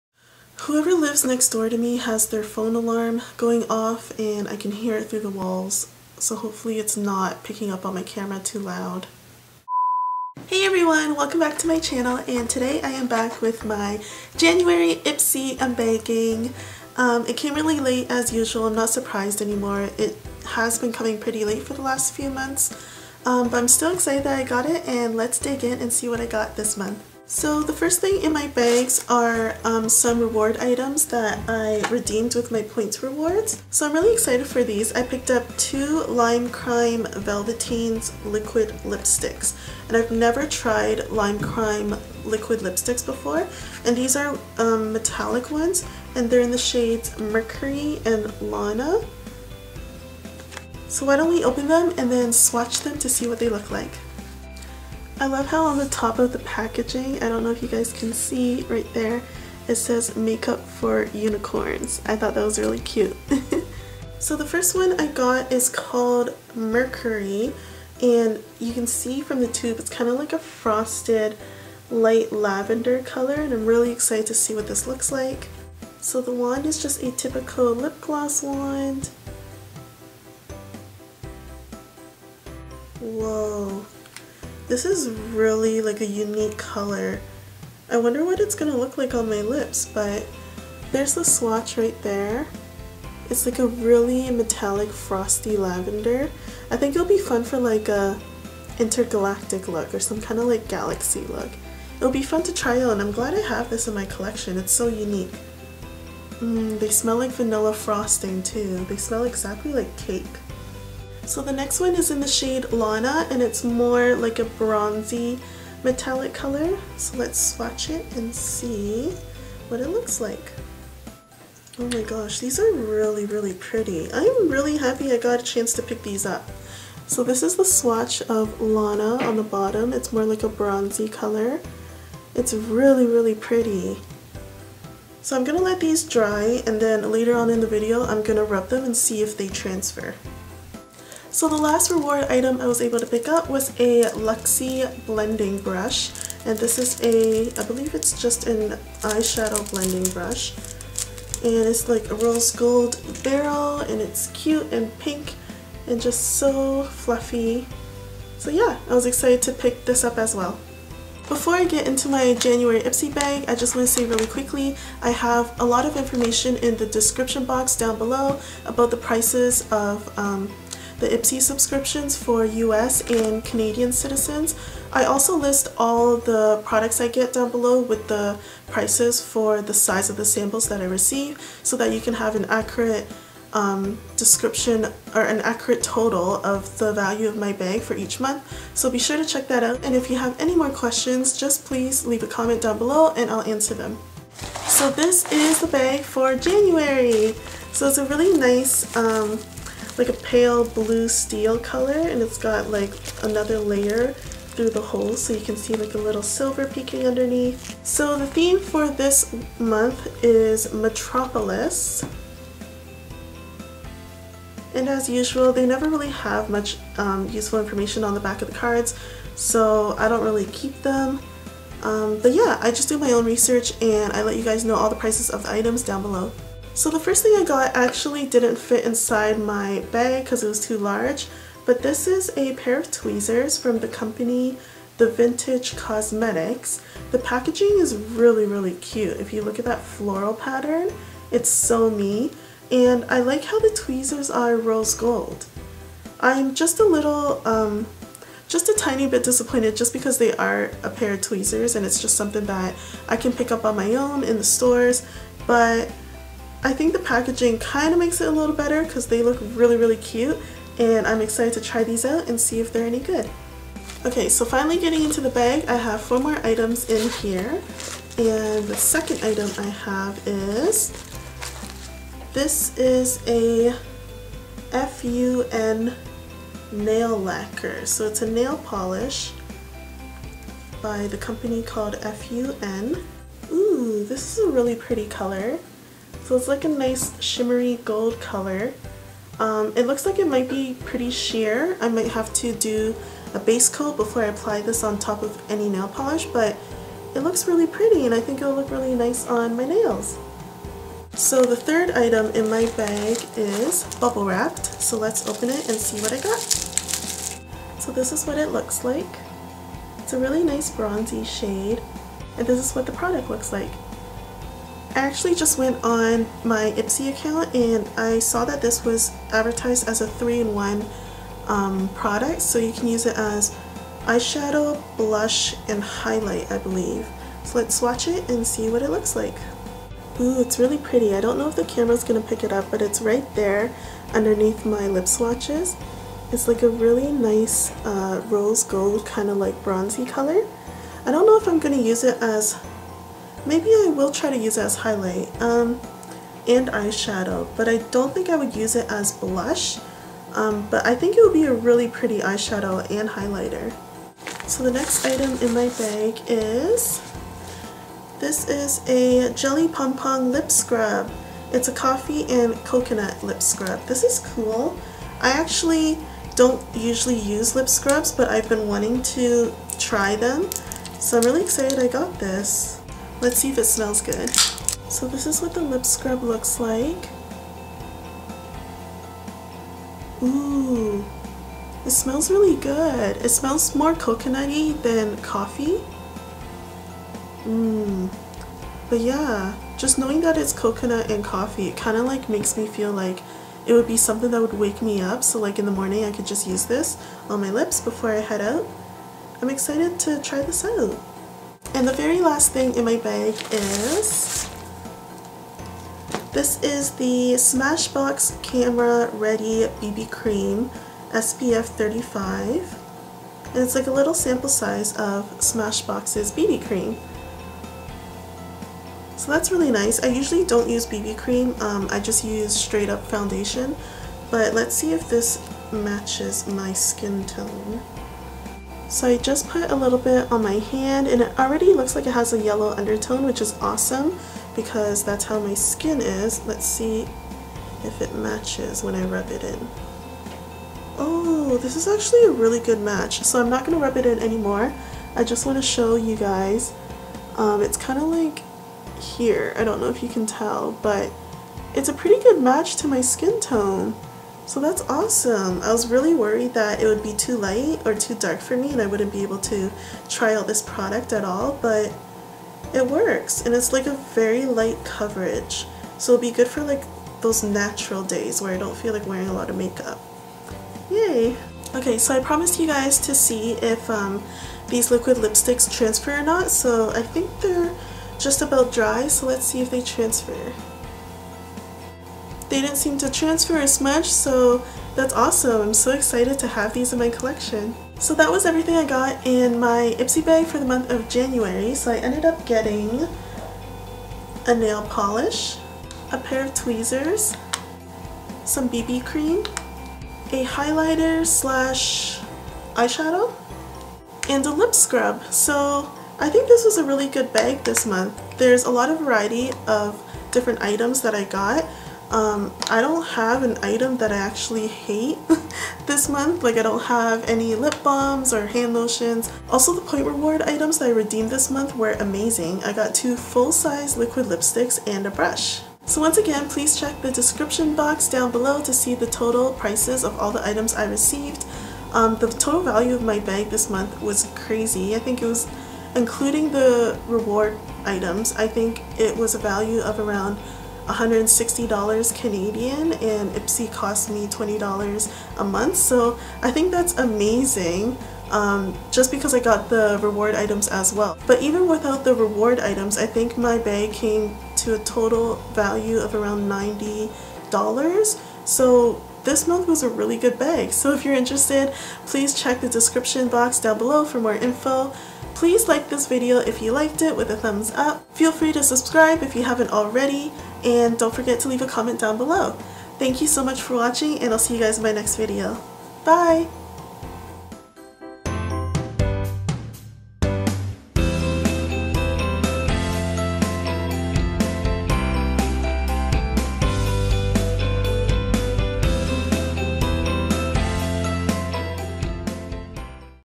ipsy January 2017 UNBAGGING + POINTS REWARDS!
ipsy subscription price:
$10/month (US)
Around $20/month (Canadian, shipping already added)

JANUARY 2017 IPSY GLAMBAG PRODUCTS & PRICES!
(ALL prices have been calculated according to the size of the products received in the bag!)

The Vintage Cosmetic Company - Rose Gold Tweezers
$9.87 (US), $13.16 (Canadian)
F.U.N. Lacquer in “TGIF”
$11.50 (US), $15.33 (Canadian)
Manna Kadar Cosmetics - "Fantasy" 3-in-1 Eyeshadow
$19.00 (US), $25.33 (Canadian)
Jelly Pong Pong - Coffee + Coconut Lip Scrub
Smashbox Cosmetics - Camera Ready BB Cream SPF 35 in “Light/Medium”
$9.75 (US), $13.00 (Canadian)

Total value for the January 2017 ipsy glambag:
$69.12 (US), $92.15 (Canadian)

Redeemed Rewards:
- Luxie Rose Gold Blending Brush 227 (value of $12.00 US/$16.00 Canadian)
- Lime Crime – Metallic Velvetine in “Lana” (value of $20.00 US/$26.66 Canadian)
- Lime Crime – Metallic Velvetine in “Mercury” (value of $20.00 US/$26.66 Canadian)

This license allows the use of this music on YouTube commercially.